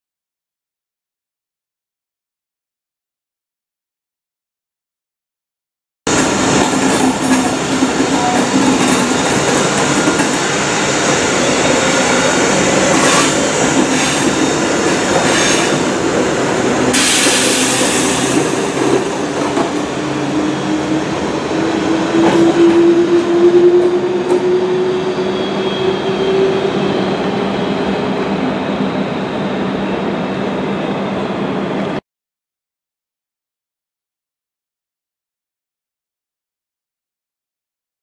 [HD] TER Local service departing Chambery
A TER local service departing Chambery for Aix Les Bains, formed of Z9500 number 9517